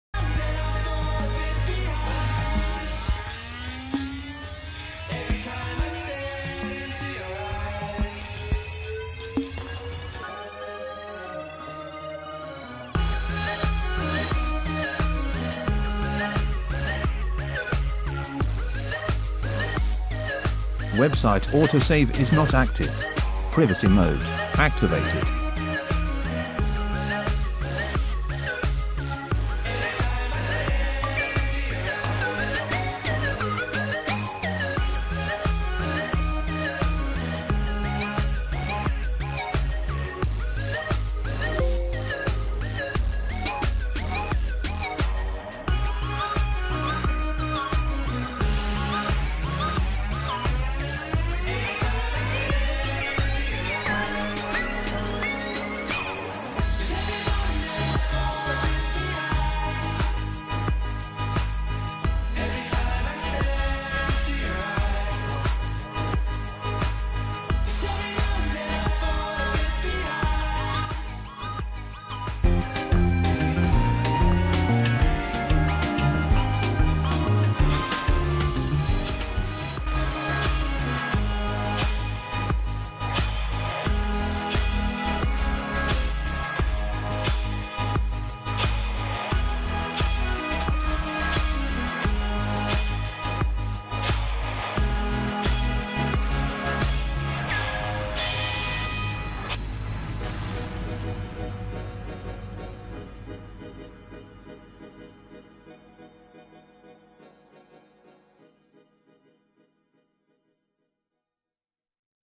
The DylNET 10 Web Browser - Functions of the future
We're very close to releasing our most complex set of software by us, DylNET 10. This video explains just some of the functions you'll find in it, and this is only a few!